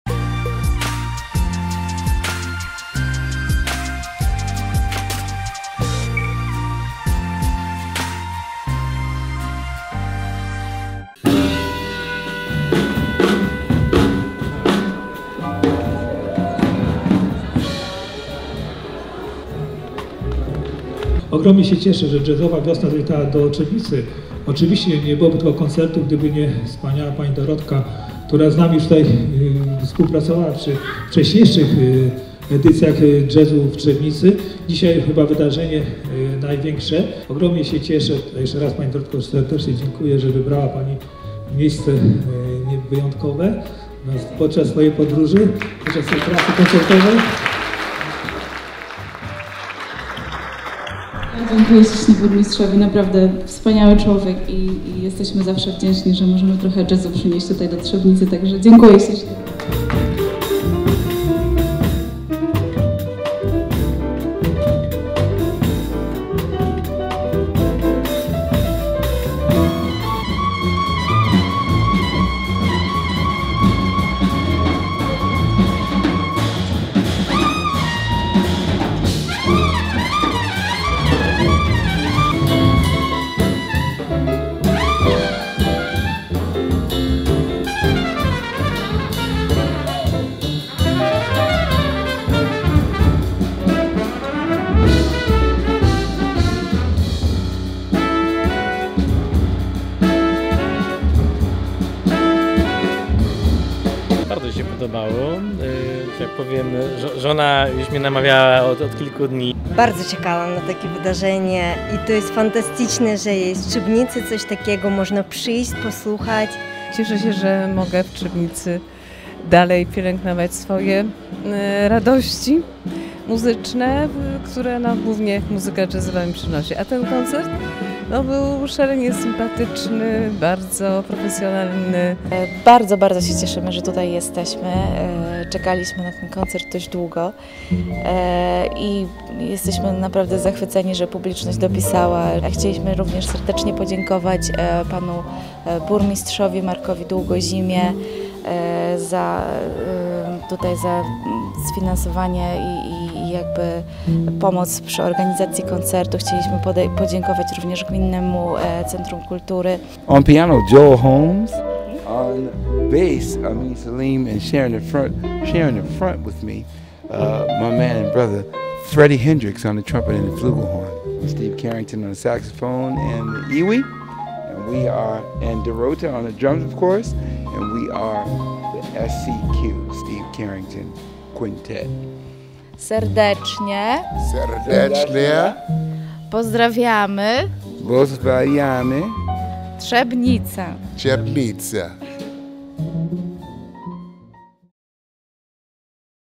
Jazzowa Wiosna w Trzebnicy
Jazzowa Wiosna zawitała do Trzebnicy w piątek 13 maja. Zapraszamy do obejrzenia relacji z koncertu, jaki odbył się w Sali Widowiskowo-Kinowej Gminnego Centrum Kultury i Sztuki